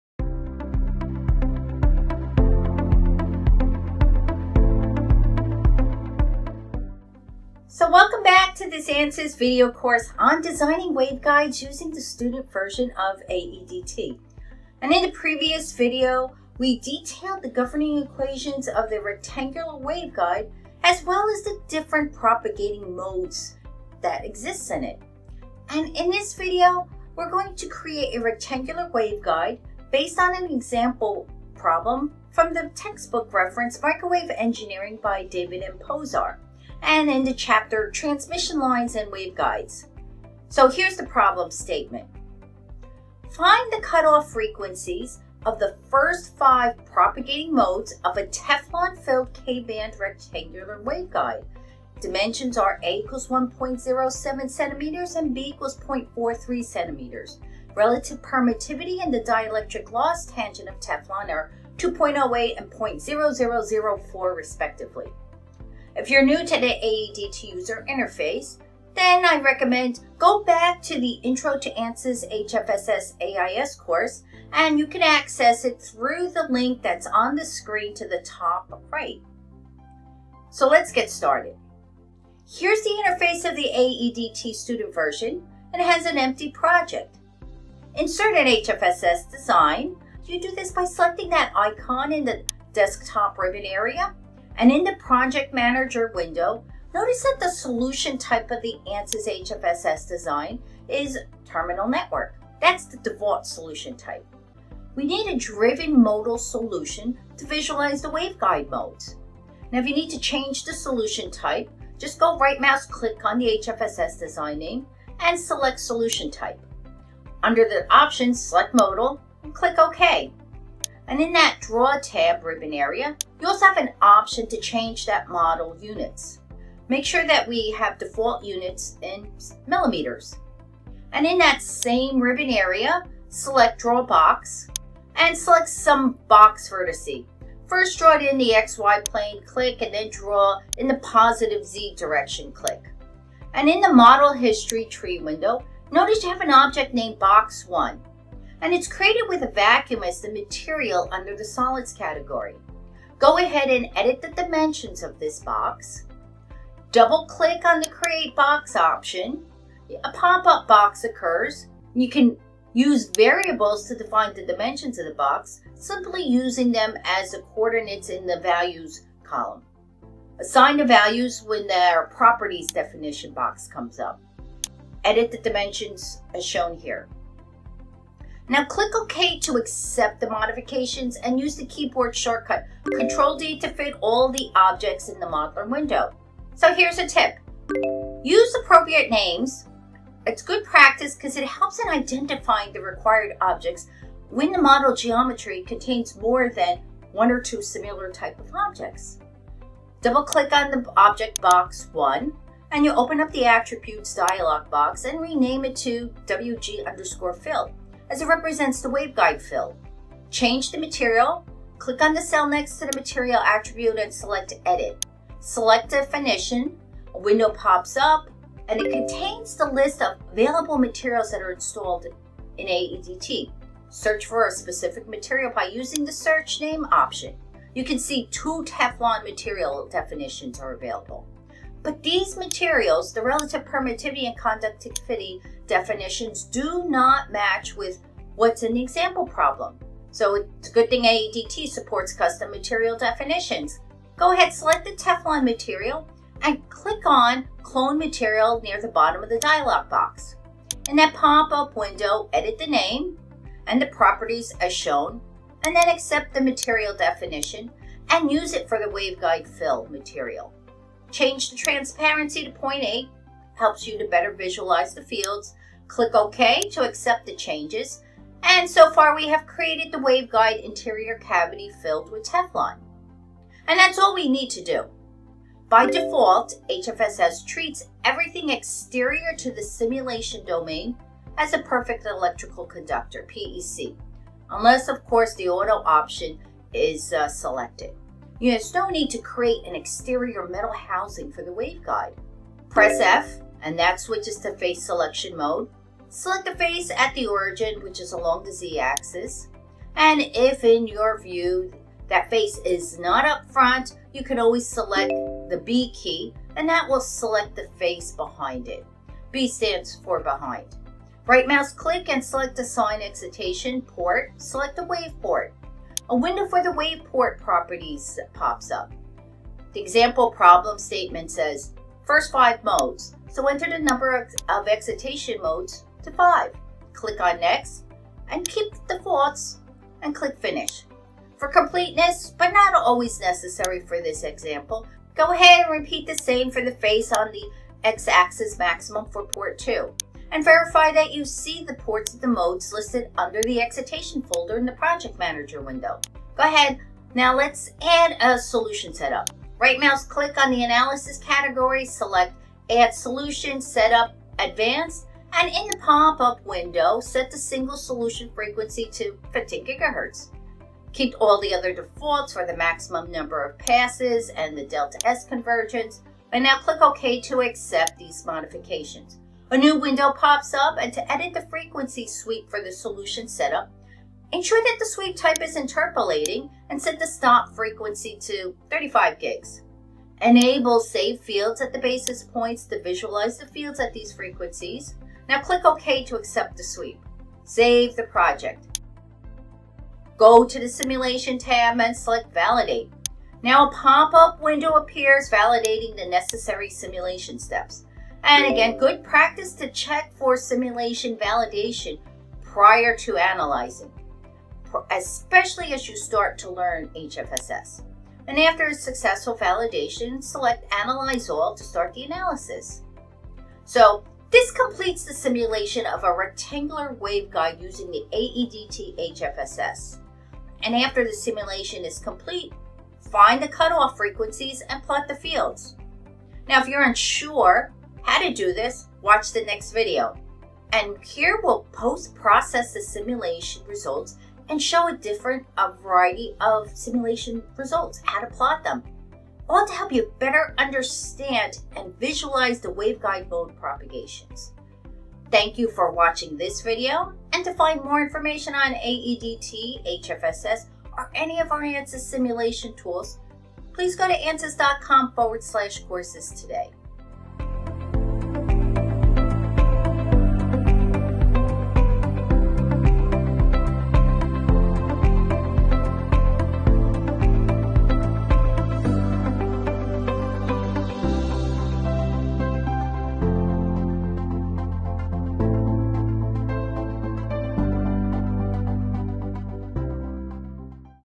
Rectangular Waveguide Simulation Using Ansys HFSS — Model Creation — Lesson 3
Rectangular Waveguide Simulation Using Ansys HFSS – Model Creation

In this video lesson, a rectangular waveguide is simulated using Ansys HFSS. This lesson covers the steps required to analyze a model in Ansys HFSS. This lesson starts with the geometry creation of the rectangular waveguide.

It shows how to use variables to define the waveguide dimensions and select and clone the specific materials available in the Ansys Electronics Desktop material library.